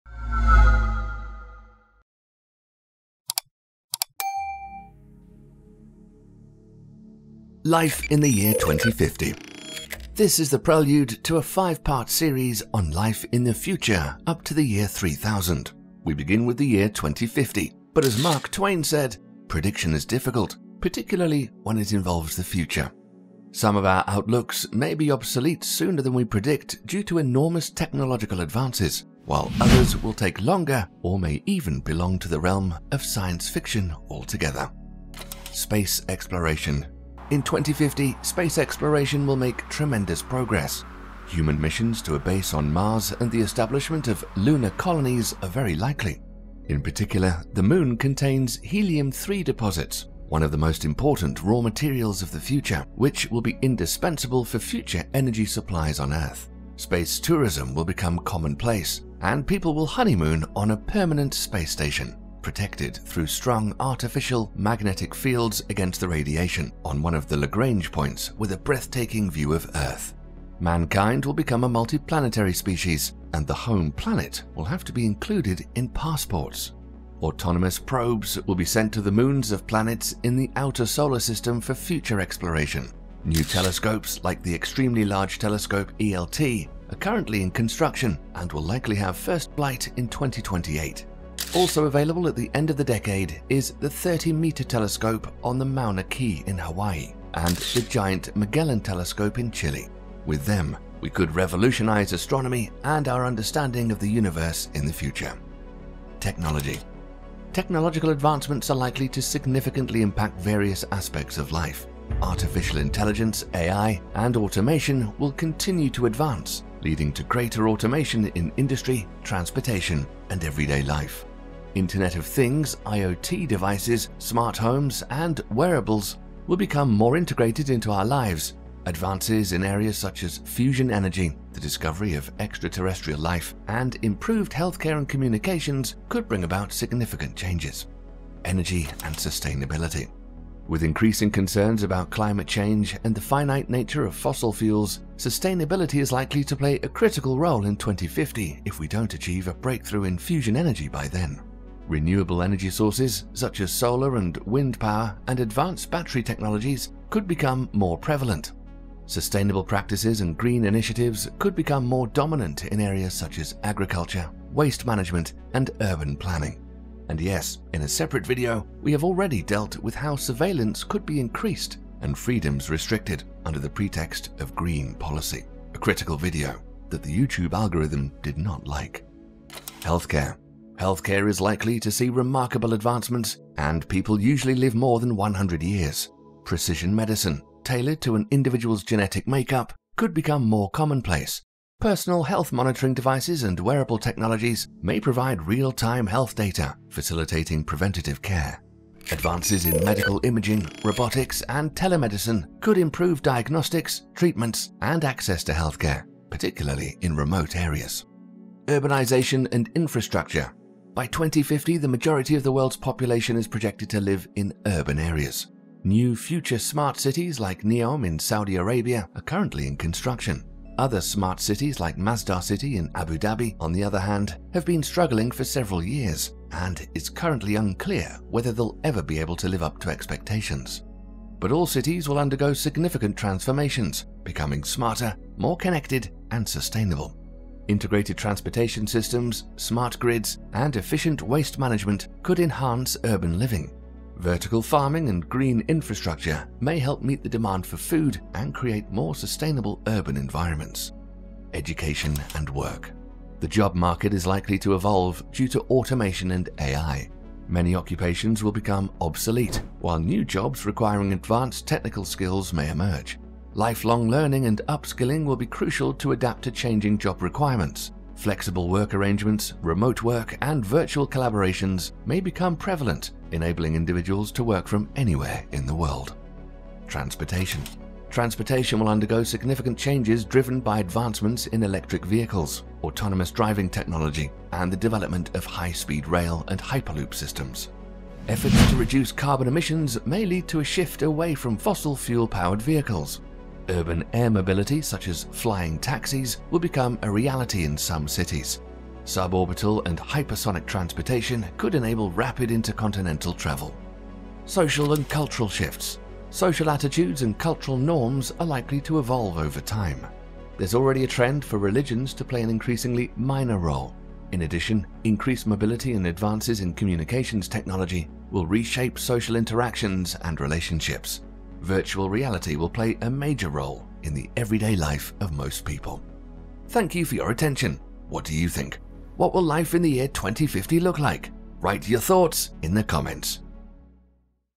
A glimpse into the future: Life in 2050
This is the prelude to a 5-part series on life in the future up to the year 3000, and we begin with the year 2050, but as Mark Twain said, "Prediction is difficult - particularly when it involves the future." Some of our outlooks may be obsolete sooner than we predict due to enormous technological progress, while others will take longer or may even belong to the realm of science fiction altogether.

Script by Sven Piper
Proofreading: Aby Jane Richards
Voice over: Tom Bennett
Editing: Sven Piper

0:00 Intro
0:35 Space Exploration
1:47 Technology
2:19 Energy & Sustainability
3:00 Healthcare
3:29 Urbanization and Infrastructure
4:12 Education and Work
4:38 Transportation
5:05 Social and Cultural shifts
5:26 Ending

Footage:
In the video used footage is from Storyblocks.com & Artgrid.io

Version 1.0